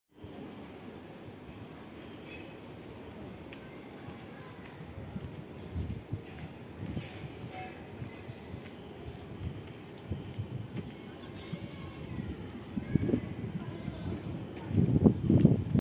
2160p 30fps (main camera) - Redmi Note 10S video sample
Video sample shot by the phone's camera. To learn more about what this smartphone is capable of, check out our full review over at GSMArena.com